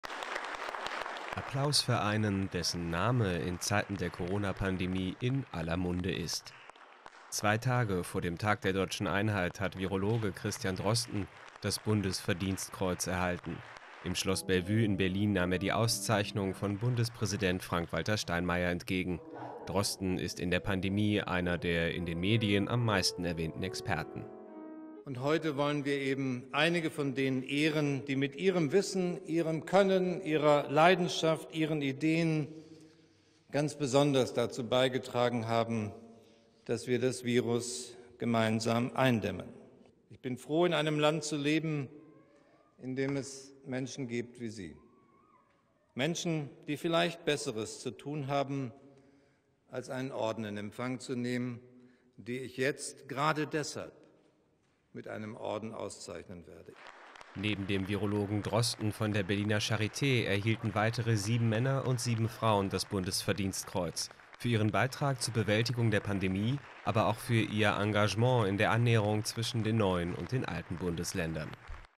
Christian Drosten erhält Bundesverdienstkreuz
Neben dem Virologen Drosten von der Berliner Charité erhielten weitere sieben Männer und sieben Frauen das Bundesverdienstkreuz – für ihren Beitrag zur Bewältigung der Corona-Pandemie, aber auch für ihr Engagement bei der Annäherung zwischen Ost und West.